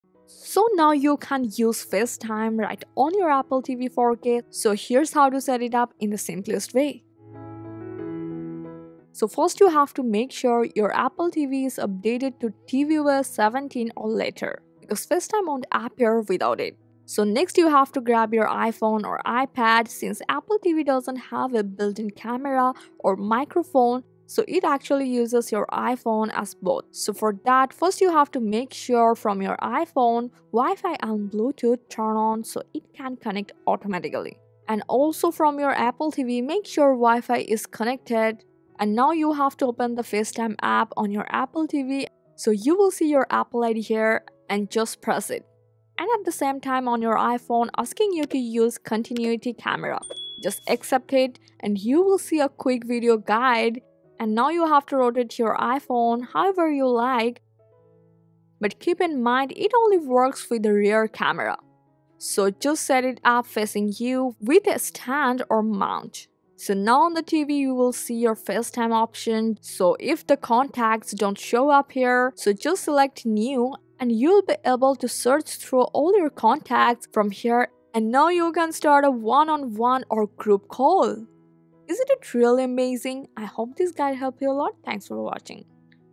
Apple TV 4K: How to Use Facetime! [Complete Setup Guide]
Now you can use FaceTime to make video calls from your Apple TV 4K running on tvOS 26, including 1st, 2nd, and 3rd Gen, want to know how?

Searching for a complete setup guide to connect FaceTime on Apple TV 4K and how to use the Mic and camera, as there is none by default? Don’t panic, the Fix369 team will walk you through the step-by-step guide to set up FaceTime on Apple TV 4K with tvOS 26 and start making videos calls using Apple TV effortlessly.

0:00 Intro: Can you use FaceTime on Apple TV 4K
0:11 Must ensure that you can use FaceTime on Apple TV 4K
0:19 Prerequisite to use FaceTime on Apple TV 4K
0:41 Setup and Use FaceTime on Apple TV 4K